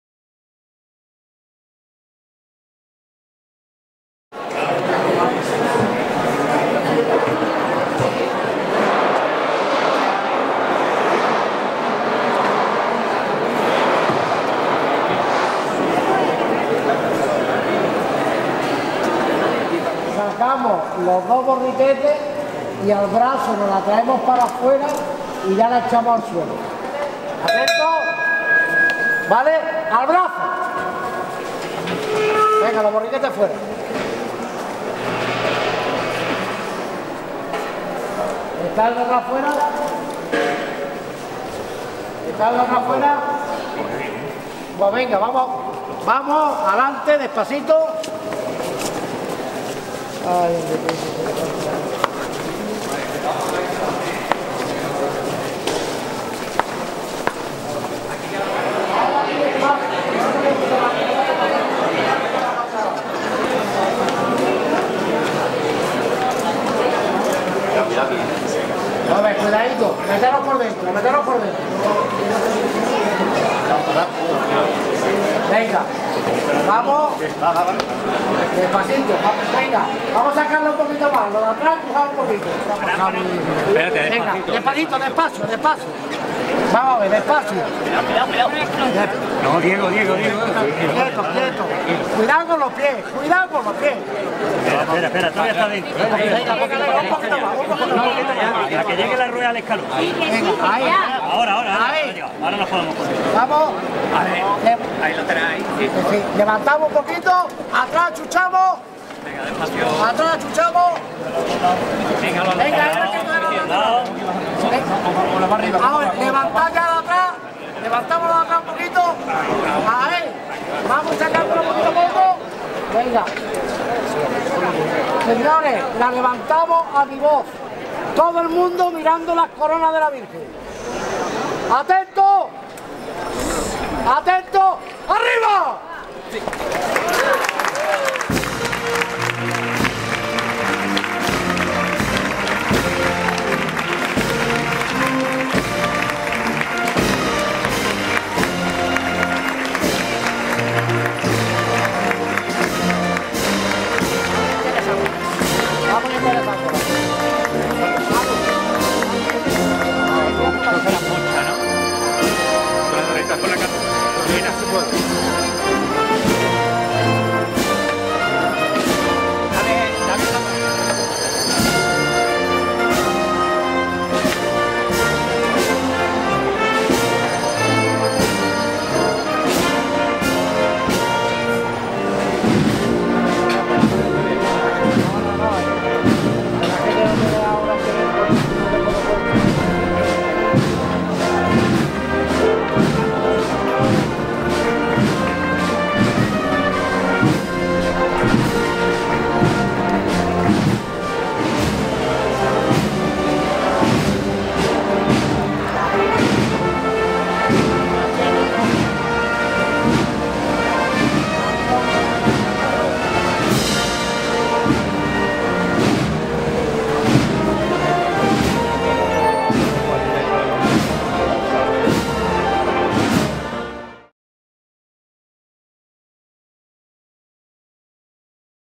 ✝️ Remedios - Salida - Antequera - 2023
✝️ Remedios - Salida - Antequera - 2023. Así fue la salida de la procesión de la Virgen de los Remedios este viernes 8 de septiembre de 2023 en Antequera. Justo Moreno Muñoz es el hermano mayor de insignia del templete de la Patrona Mariana de Antequera.